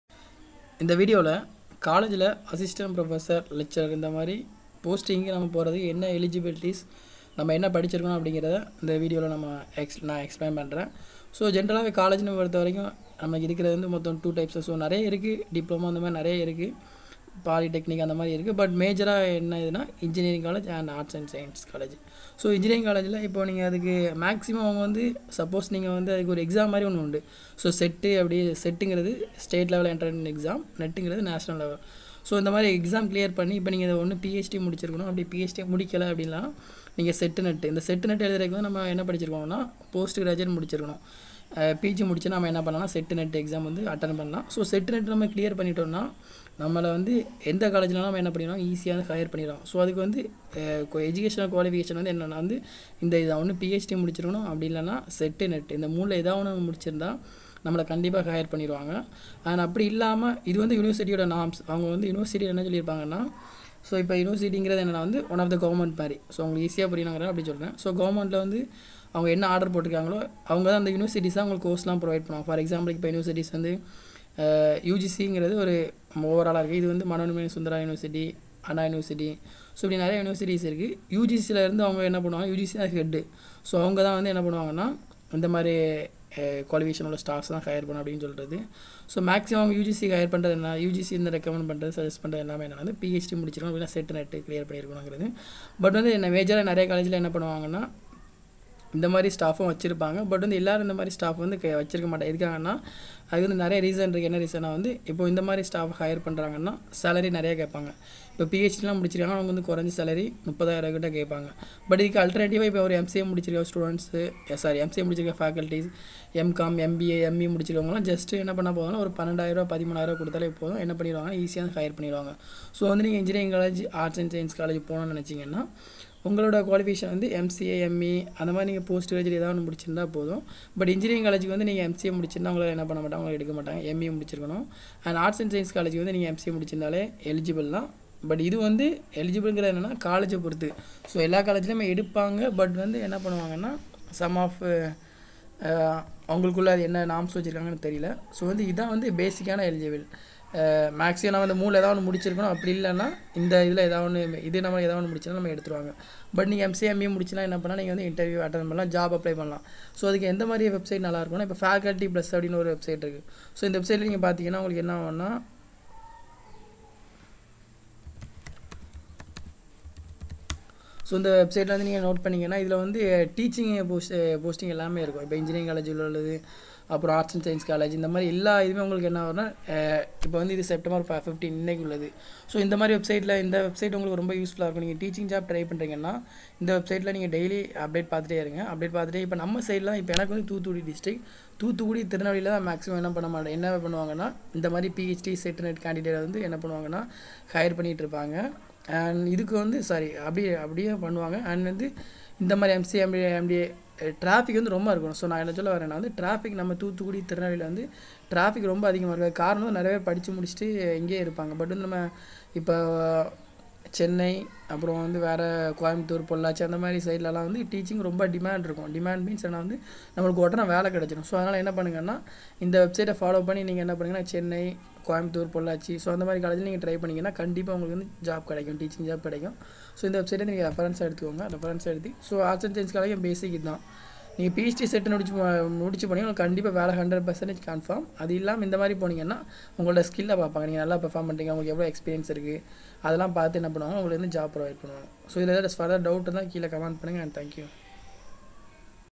How to become a Assistant Professor/Lecturer | Job Tips | Eligibility | Tamil
here I explained how become a assistant professor and lecturer in both of arts and science college, engineering in tamil. and also explained how get jobs.